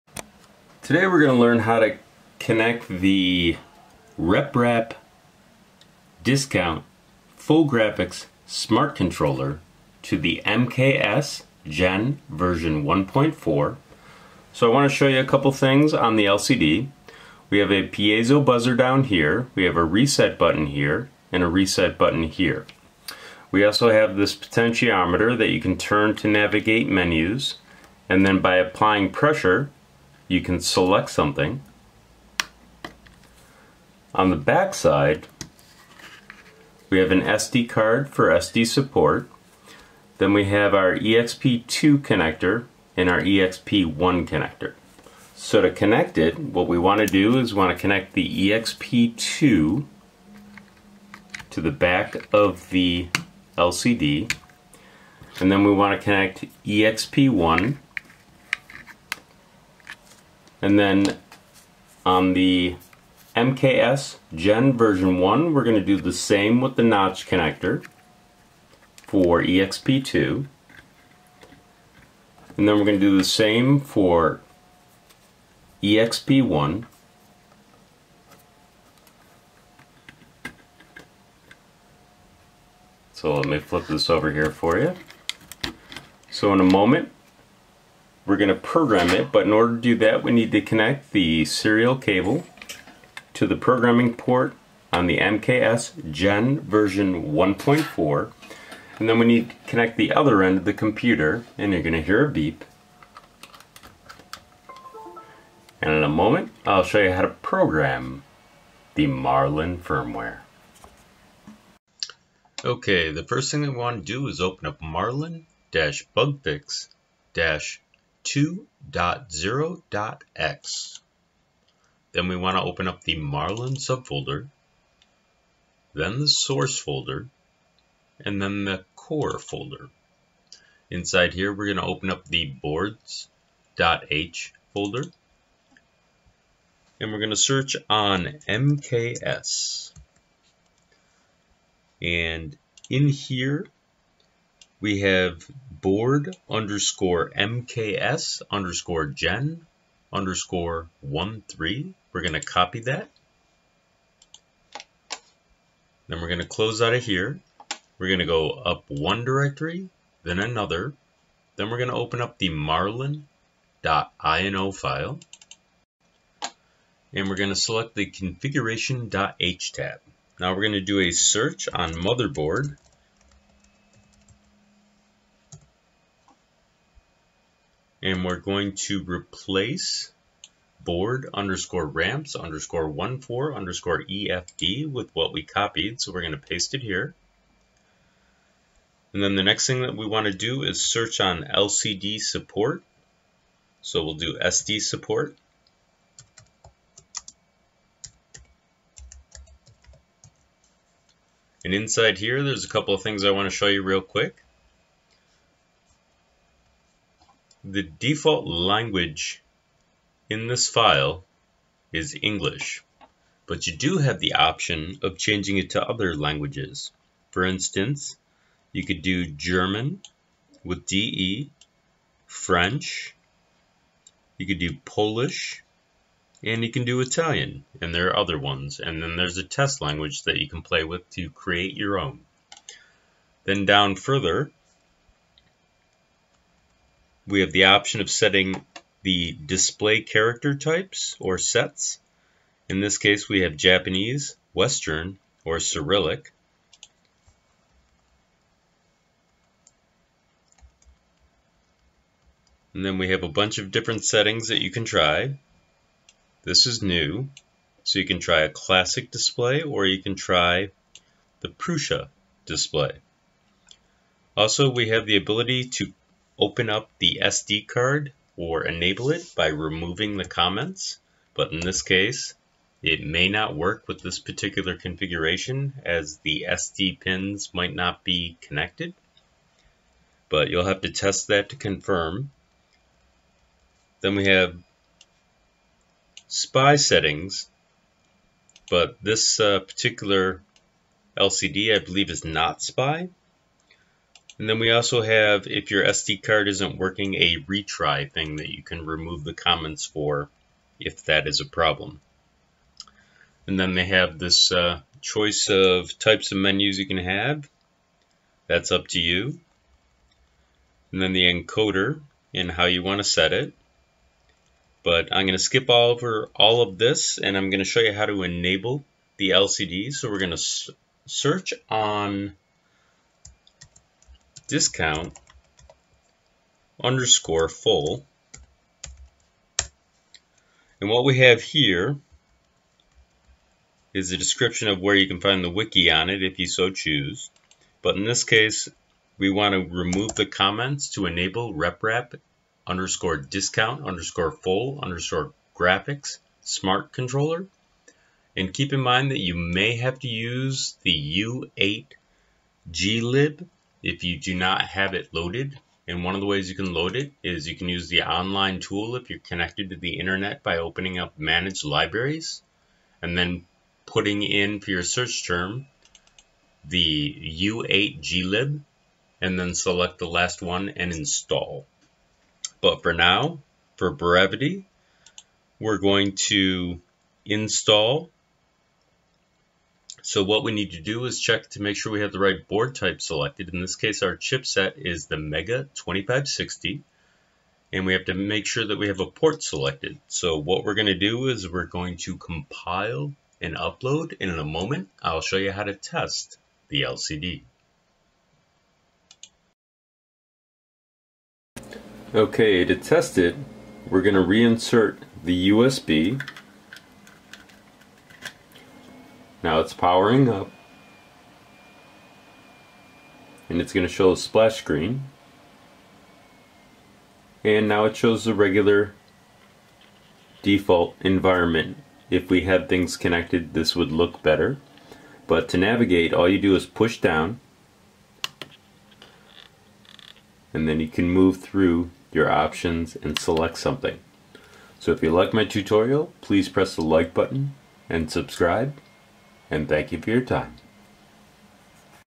MKS Gen 1.4 - DISCOUNT FULL GRAPHIC SMART CONTROLLER
This is a tutorial on how to add an  RepRap discount full graphics smart controller LCD on the MKS Gen 1.4 3d printer board.

Required hardware:
MKS Gen 1.4 board
USB Cable
2864 LCD Display Controller Adapter

Required software:
Arduino IDE (version 1.8.7 or higher)